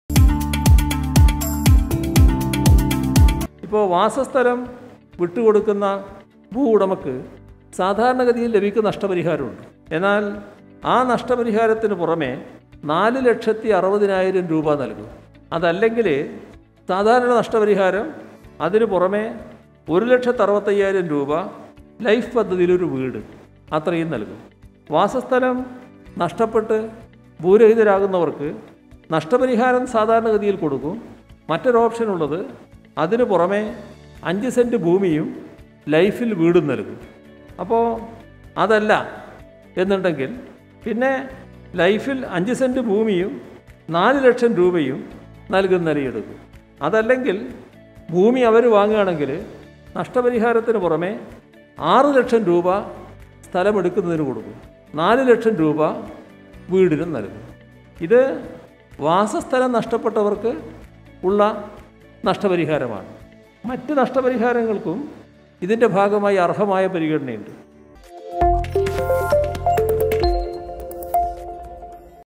സില്‍വര്‍ ലൈന്‍ പദ്ധതി , വാസസ്ഥലം നഷ്ടമാകുന്നവര്‍ക്ക് സാധാരണ നഷ്ടപരിഹാരത്തിന് പുറമേ 4,60,000 രൂപ
സില്‍വര്‍ലൈന്‍ പദ്ധതിക്ക് വേണ്ടി ഭൂമി ഏറ്റടുക്കുമ്പോള്‍ വാസ സ്ഥലം നഷ്ടമാകുന്നവര്‍ക്ക് സാധാരണ നഷ്ടപരിഹാരത്തിന് പുറമേ 4,60,000 രൂപ നല്‍കും.അല്ലെങ്കില്‍ സാധാരണ നഷ്ടപരിഹാരവും അതിനു പുറമേ 1,65,000 രൂപ,ലൈഫ് പദ്ധതിയില്‍ ഒരു വീട് അത്രയും
നല്‍കും. വാസസ്ഥലം നഷ്ടപ്പെട്ട് ഭൂരഹിതരാവുന്നവര്‍ക്ക് നഷ്ടപരിഹാരം സാധാരണ ഗതിയില്‍ നല്‍കും.അതല്ലെന്നുണ്ടെങ്കില്‍ ലൈഫില്‍
അഞ്ച് സെന്റ് ഭൂമിയും നാല് ലക്ഷം രൂപയും നല്‍കുന്നതായിരിക്കും.അതും അല്ലെങ്കില്‍ ഭൂമി അവര്‍ വാങ്ങുകയാണെങ്കില്‍ നഷ്പരിഹാരത്തിന് പുറമേ 6 ലക്ഷം രൂപ സ്ഥലം എടുക്കുന്നതിന് നല്‍കും.നാല് ലക്ഷം രൂപ വീടിനും നല്‍കും.ഇത് വാസസ്ഥലം നഷ്ടപ്പെട്ടവര്‍ക്കുളള നഷ്ടപരിഹാരമാണ്.സില്‍വര്‍ ലൈനിനു വേണ്ടി ഭൂമി ഏറ്റെടുക്കുമ്പോള്‍ മറ്റ് നഷ്ടം സംഭവിക്കുന്നവര്‍ക്കും അര്‍ഹമായ നഷ്ട പരിഹാരം നല്‍കും.അതിന്റെ കൂടെ 50,000രൂപയും നല്‍കും.വാസസ്ഥലം നഷ്ടപ്പെടുന്ന വാടക താമസക്കാര്‍ക്ക് 30,000 രൂപ നല്‍കും.
ഭൂമി ഏറ്റെടുക്കുമ്പോള്‍ ഏറ്റെടുക്കേണ്ടി വരുന്ന വാണിജ്യ സ്ഥാപനങ്ങളിലെ തൊഴിലാളികള്‍ക്ക് 6000 രൂപ വീതം ആറ് മാസം
നല്‍കും.നിലവിലെ കണക്കുകള്‍ പ്രകാരം പദ്ധതിക്ക് 1383 ഹെക്ടര്‍ ഭൂമിയാണ് പുനരധിവാസമുള്‍പ്പെടെയുളള ആവിശ്യങ്ങള്‍ക്കായി വേണ്ടി വരുന്നത്.